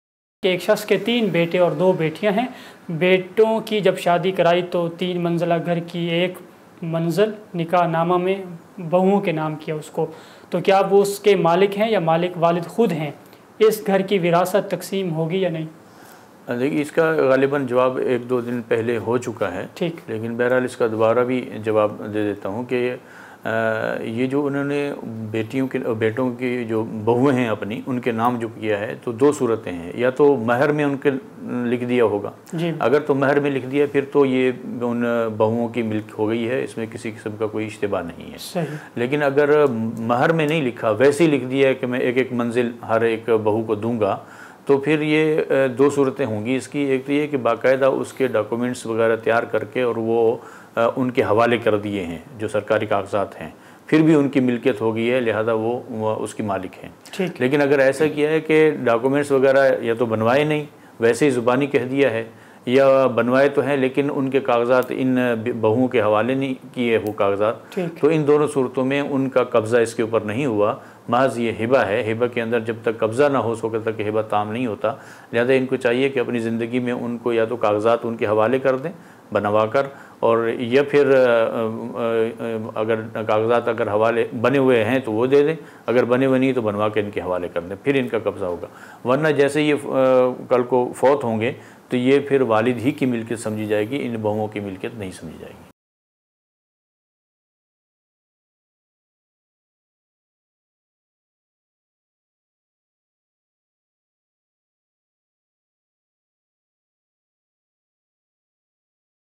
Makan Mehar Main Likh Dia To Milke Ban Gai Ya Nahi ? | Jamia-Tur-Rasheed Pakistan
Makan Mehar Main Likh Dia To Milke Ban Gai Ya Nahi ?
مکان مہر میں لکھ دیاتوملک بن گئی یانیں؟
 Mufti Asadullah Shahbaz Sb
 مفتی اسداللہ شھباز صاحب
استاذجامعۃ الرشیدکراچی